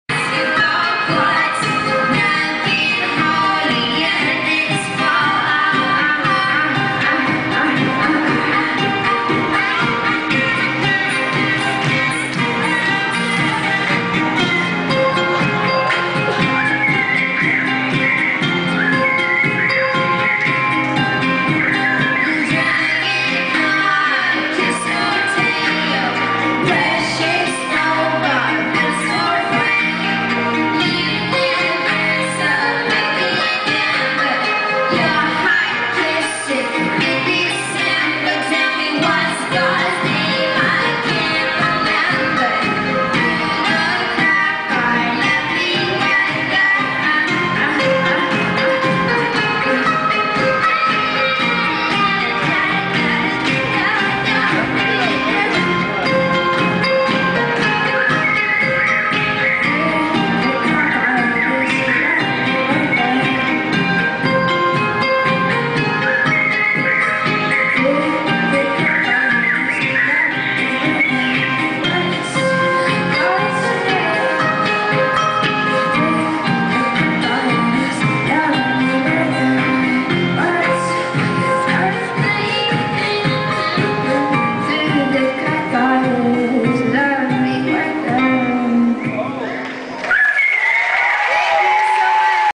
Cocorosie em Recife
Cocorosie tocando no Festival Coquetel Molotov, Recife!